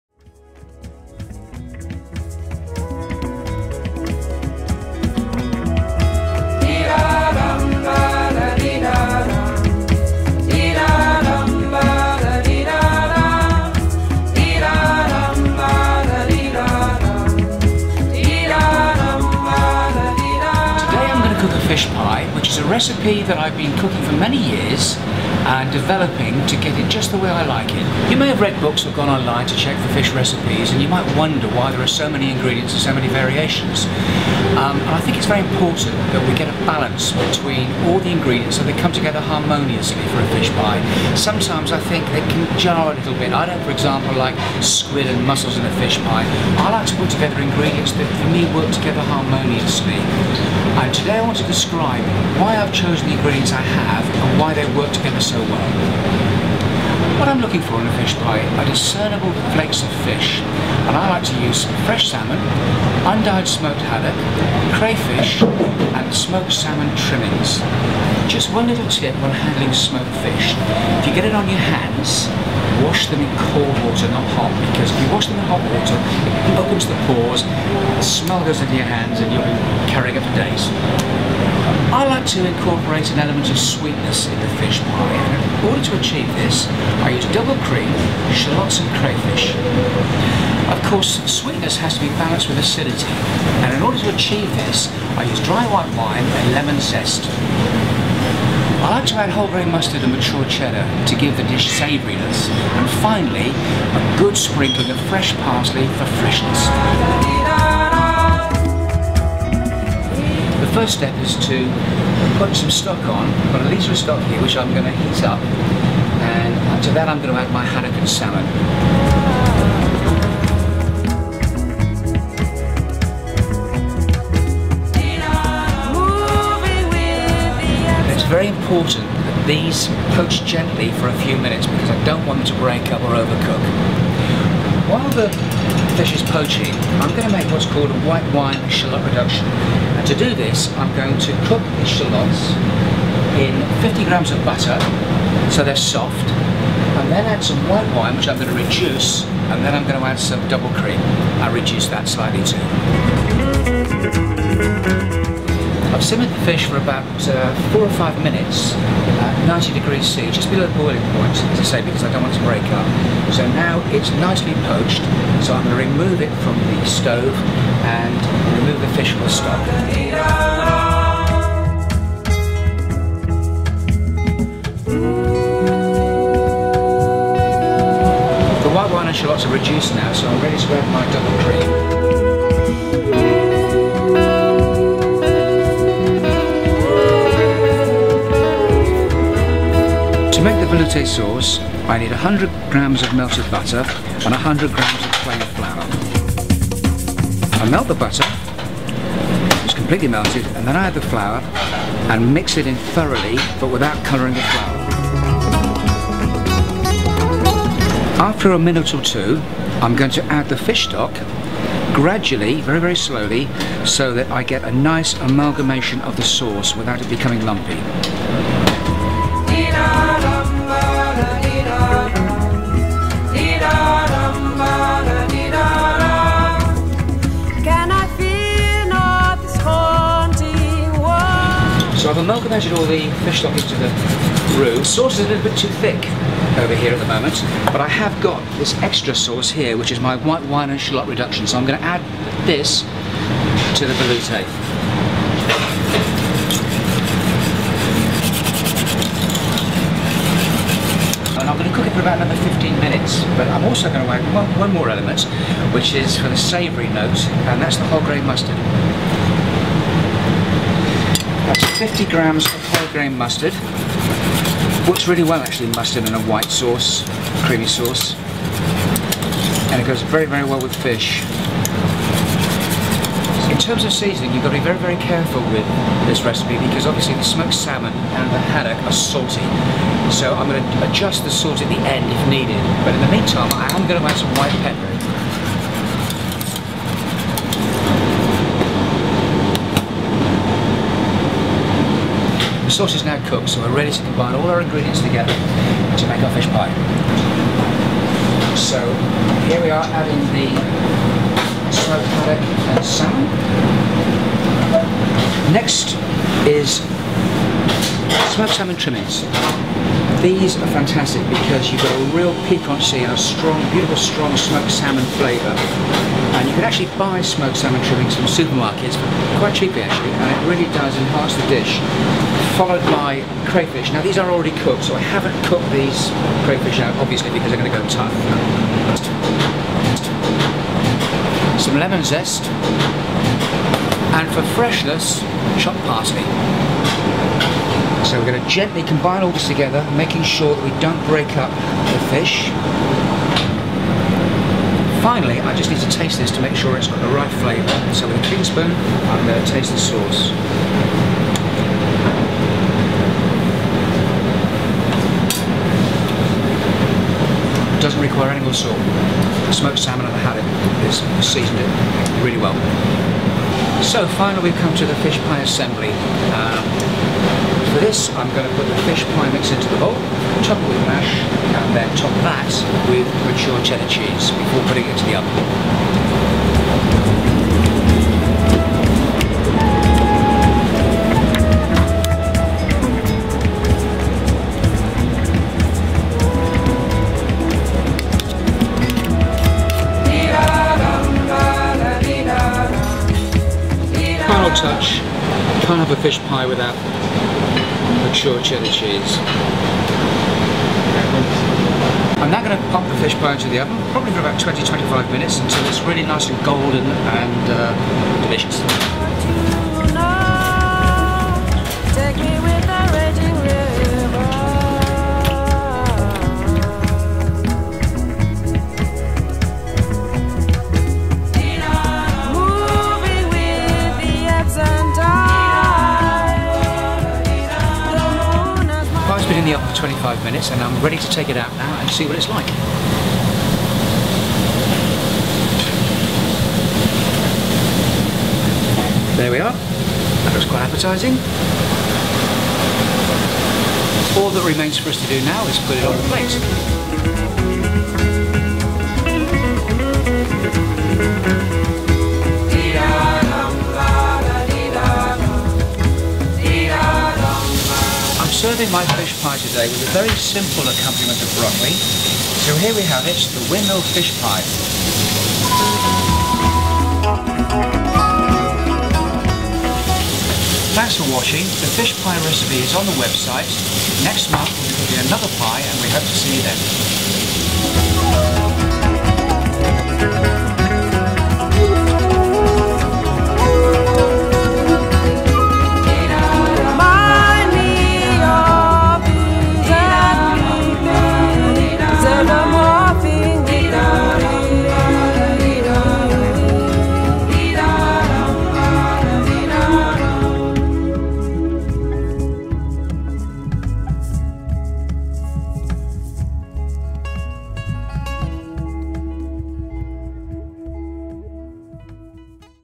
Fish Pie monthly recipe at The Windmill
This month's recipe - Fish Pie, cooked in the award winning kitchens of The Windmill, Mayfair by Pie Master Carl Smith